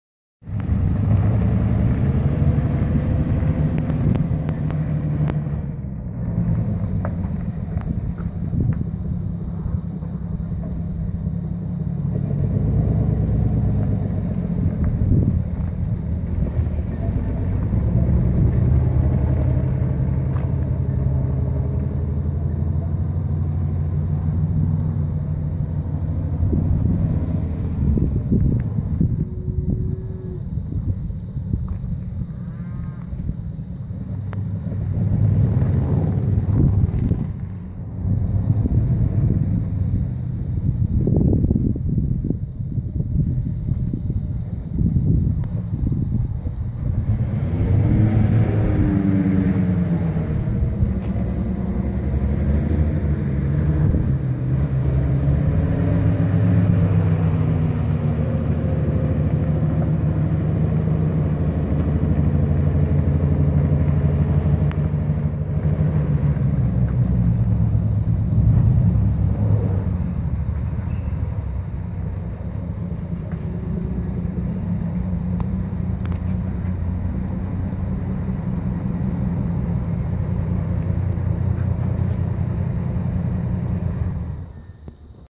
International Travelette lot drive.
1st time this truck has been run or driven in about 10 years. Basic tune up parts, some good gas, and a little bit of love.